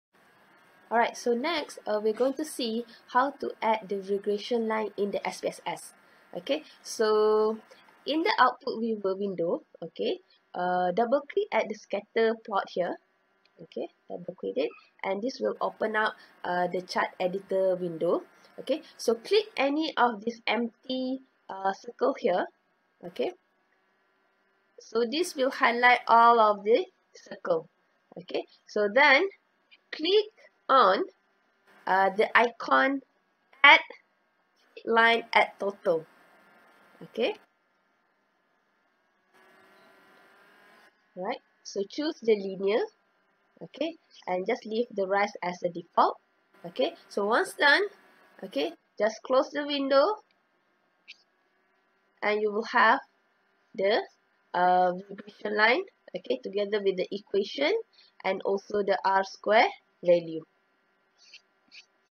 How to Add Regression Line in SPSS?
This video will shows the simple steps in adding regression line in scatterplot